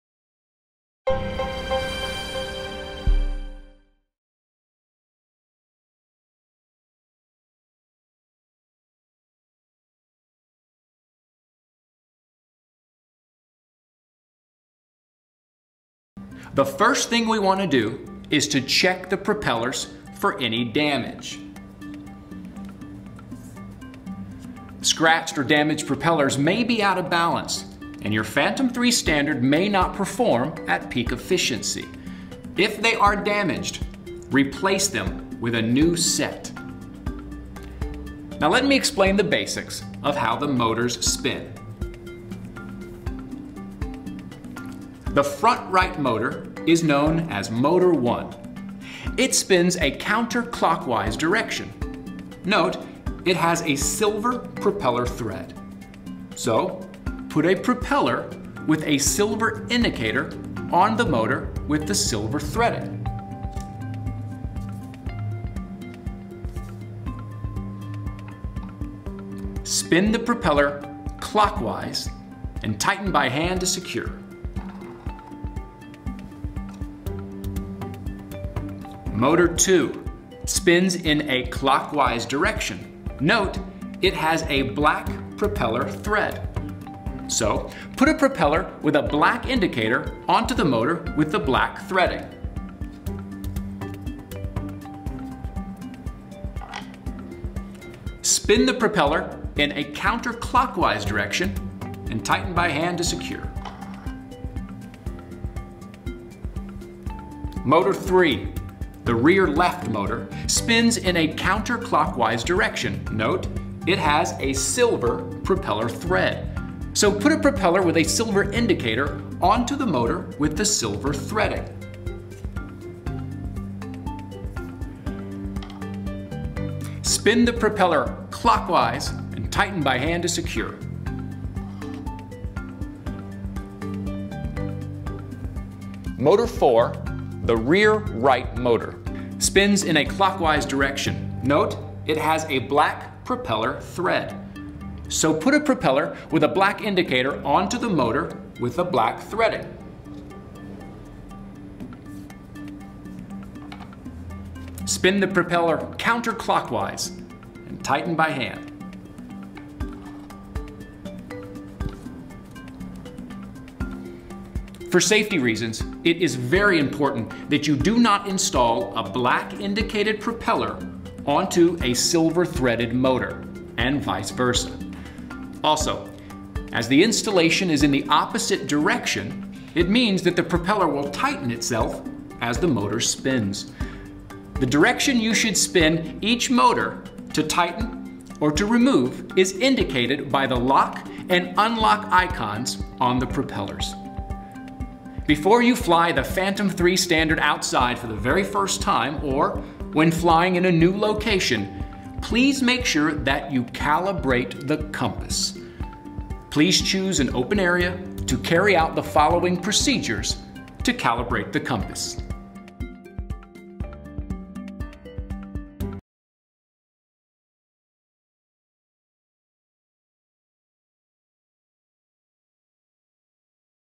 DJI Tutorials - Phantom 3 Standard - Installing the Propellers
In this video, we show you check your Phantom 3 Standard’s propellers, and how to mount them onto the aircraft before flying. For safety reasons, it is important that you install the propellers correctly onto the motors.